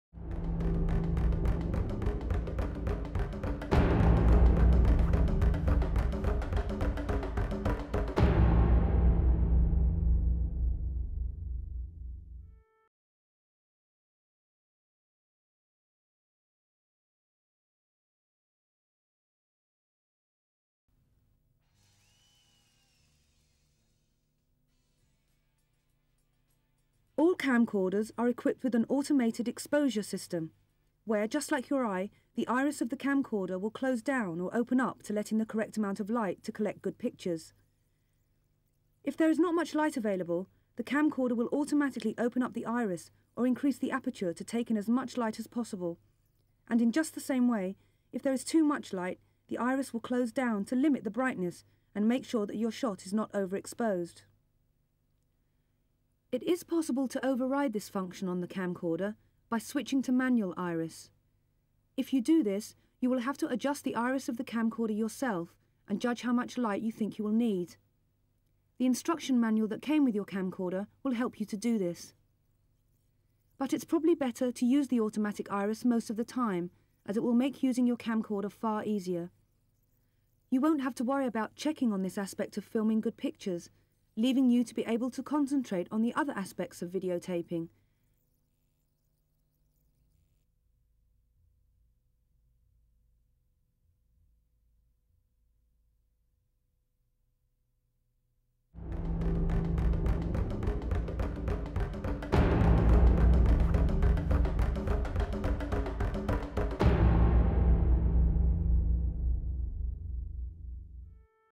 Exposure
| All camcorders are equipped with an automated exposure system, where just like your eye, the iris of the camcorder will close down or open up to let in the correct amount of light to collect good pictures.

Witness.org